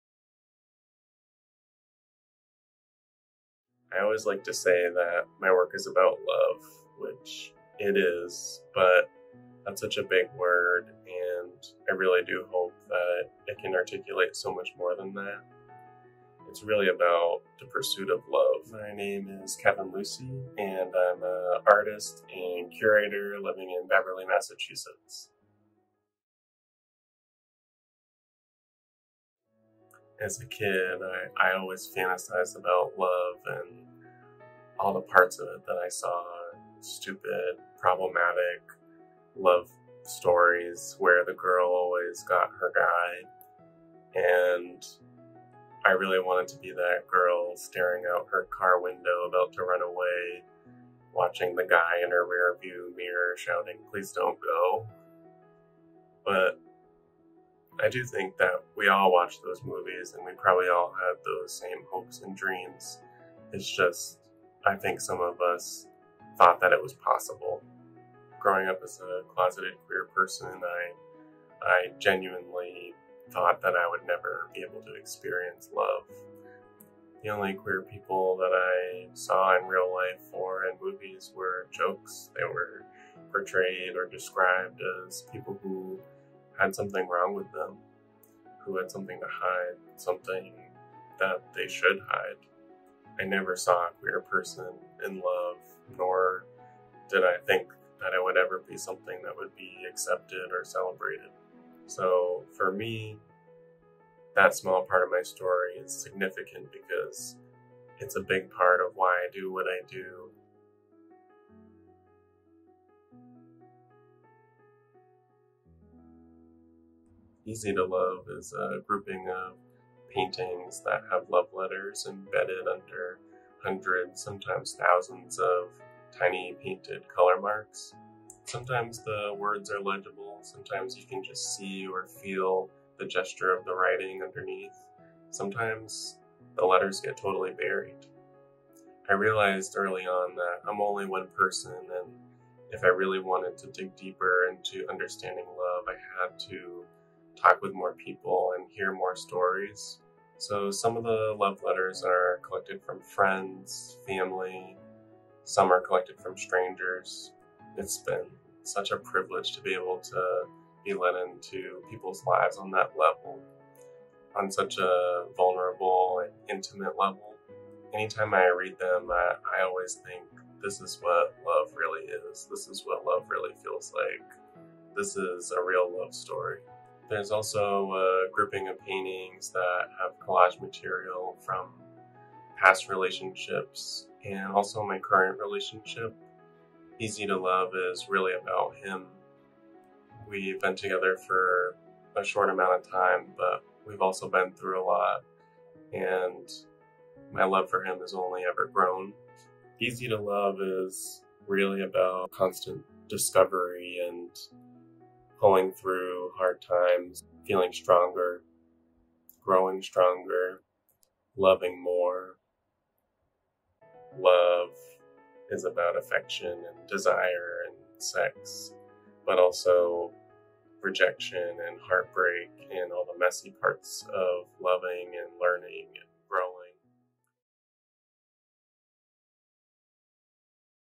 Kevin Lucey
Kevin Lucey is our next artist attracting attention in our virtual gallery. Kevin is an artist and curator who lives and works in Beverly, Massachusetts. He received a BFA with a concentration in Painting from Montserrat College of Art in 2015 and will receive a MPS in UX Design from Maryland Institute College of Art in 2021. Currently, he is the Assistant Director of Exhibitions and Public Programs at Montserrat College of Art. Lucey has curated exhibitions at A R E A (Boston, MA), The Distillery Gallery (Boston, MA), Boston Center for the Arts (Boston, MA), 222 Cabot (Beverly, MA), and Montserrat College of Art (Beverly, MA). He has exhibited his work in numerous galleries throughout the U.S., such as: Kendall College of Art and Design (Grand Rapids, MI), The University of Southern Mississippi (Hattiesburg, MS), Mingo Gallery (Beverly, MA), and Jane Deering Gallery, (Gloucester, MA).